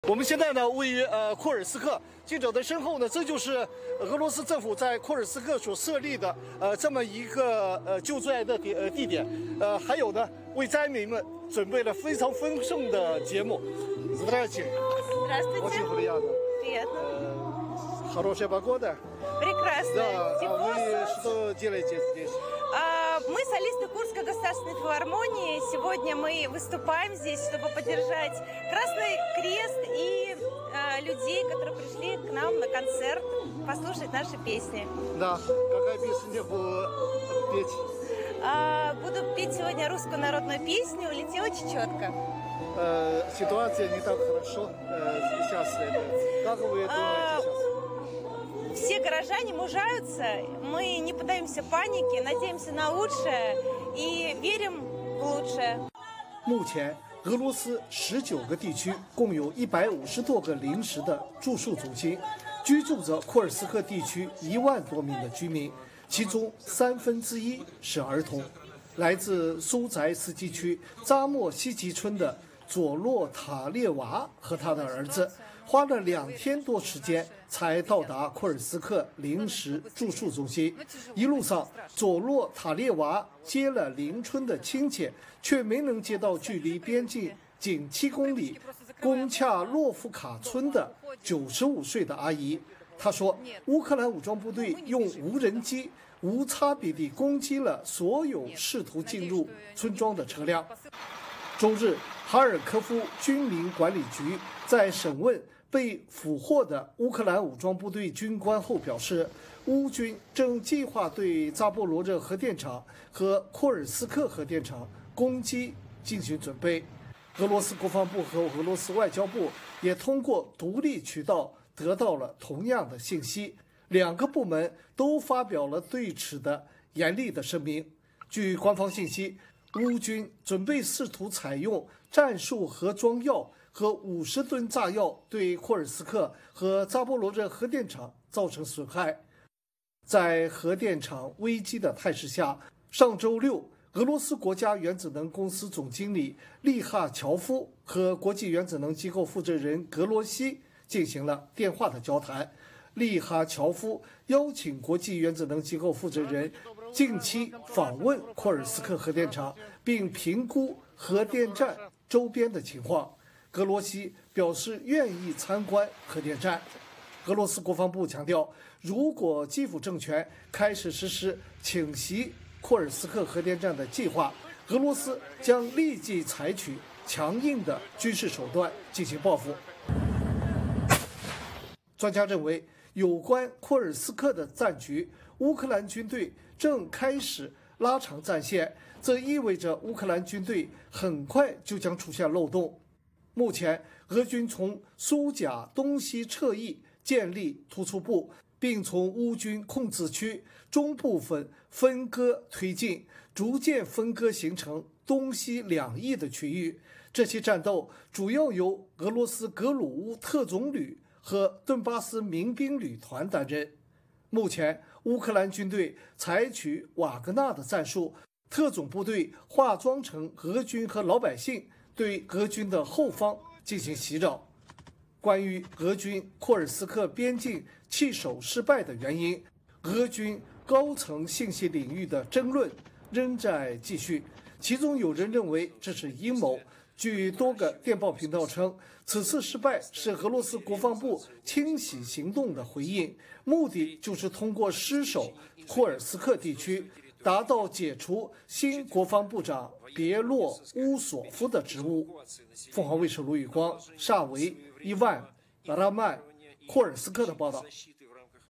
【俄烏戰局】烏軍化整為零襲擾俄軍後方 被指計劃攻擊核電站 核電站危機亮起紅燈｜盧宇光前方報道｜鳳凰衛視
俄軍當地時間18日表示，目前庫爾斯克州的戰區邊界並不明確，烏克蘭特種部隊以小組形式深入俄軍後方，進行襲擾活動。鳳凰衛視記者盧宇光從庫爾斯克前線，發回現場報道。